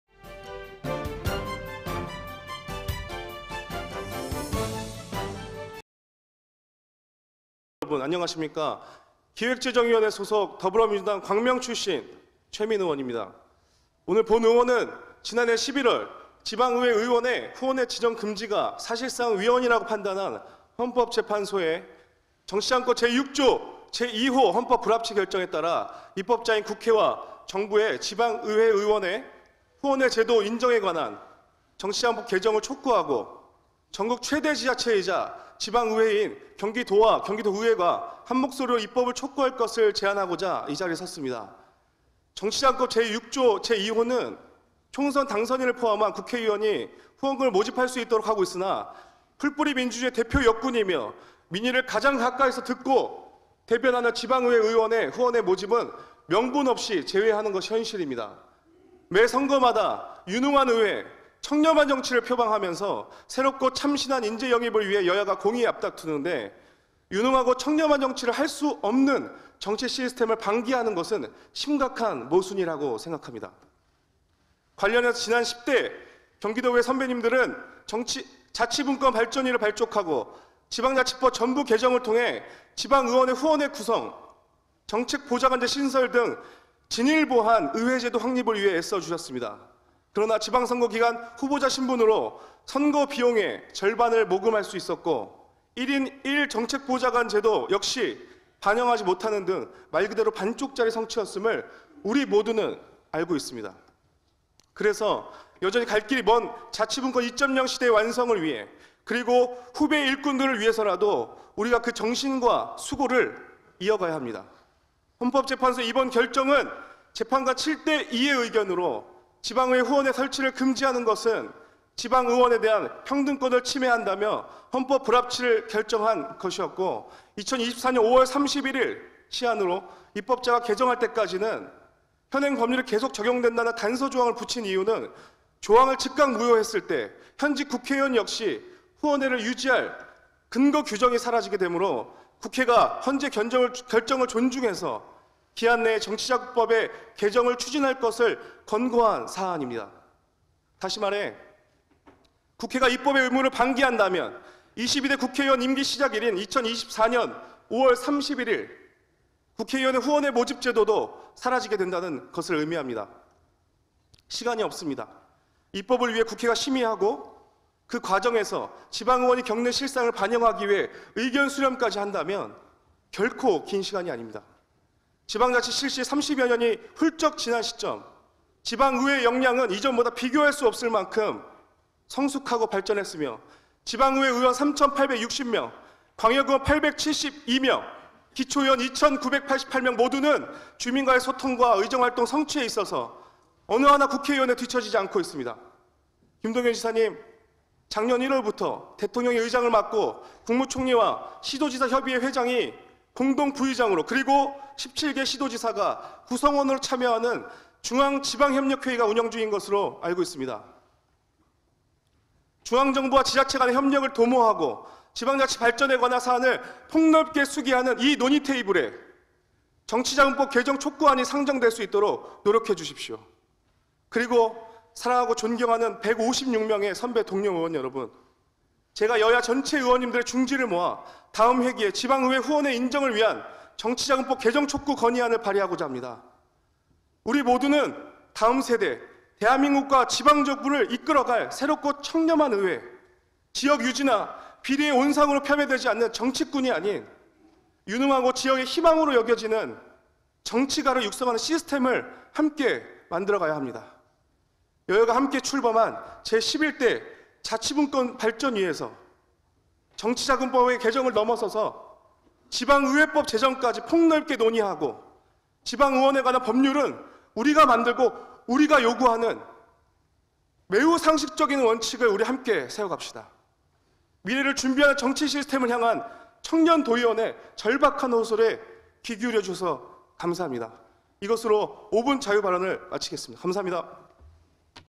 지방의회의원 후원회 제도 관련정치자금법 개정 강력 촉구
[양해용 기자]=헌법재판소의 이번 결정은 재판관 7대 2의 의견으로 지방의원 후원회 설치를 금지하는 것은 지방의원에 대한 평등권을 침해한다며 헌법불합치를 결정한 것이었고,

2024년 5월 31일 시한으로 입법자가 개정할 때까지는 현행 법률이 계속 적용된다는 단서 조항을 붙인 이유는 조항을 즉각 무효로 만들었을 때 현직 국회의원 역시 후원회를 유지할 근거 규정이 사라지게 되므로, 국회가 헌재 결정을 존중해서 기한 내에 정치자금법의 개정을 추진할 것을 권고한 사안입니다.